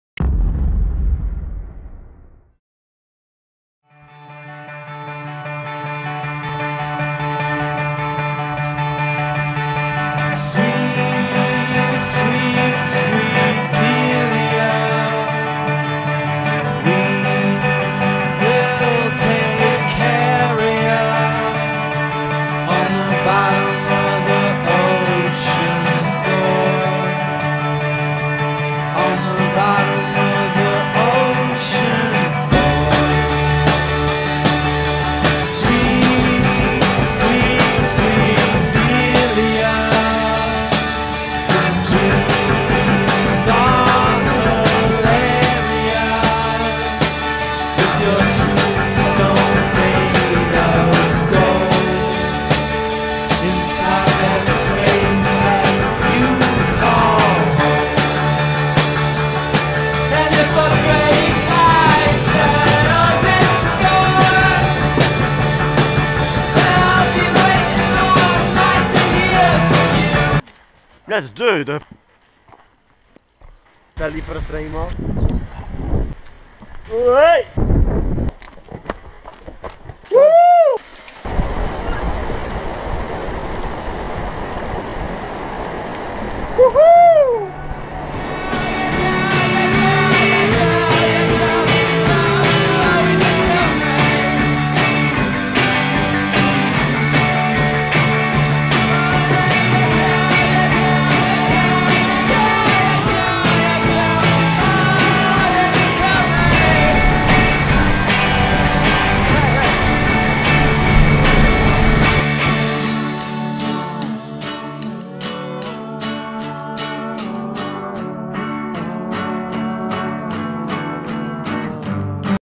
Lagoa do Fogo - Lombadas - Trail
Trilho que vai desde o miradouro da Lagoa do Fogo até ás Lombadas feito na companhia do meu amigo José Wallenstein.